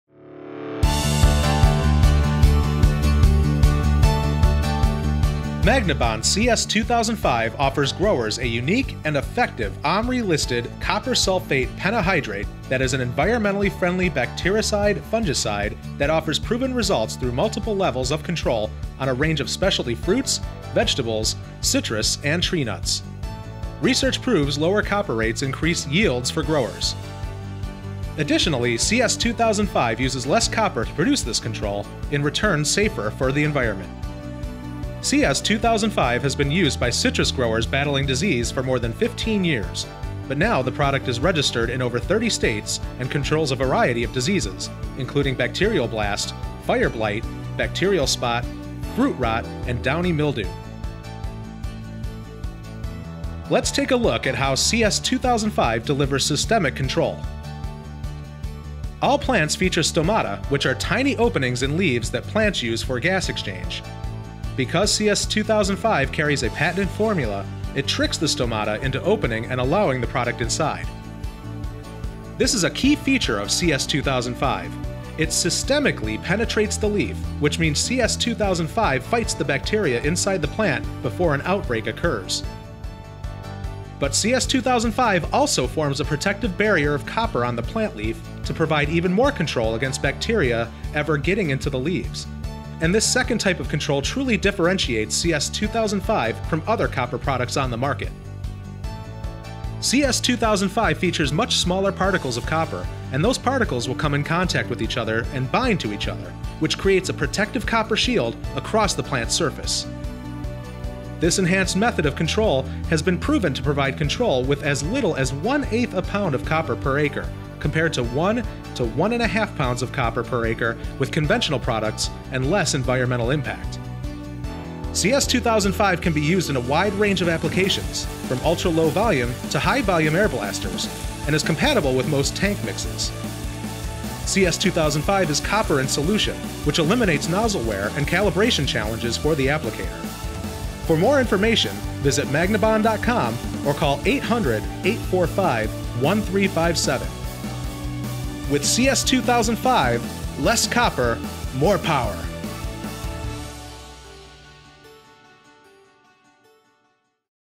MagnaBon CS 2005
Magna-Bon CS 2005 video describes how our OMRI-listed copper sulphate pentahydrate in-solution formula can help you fight agriculture disease!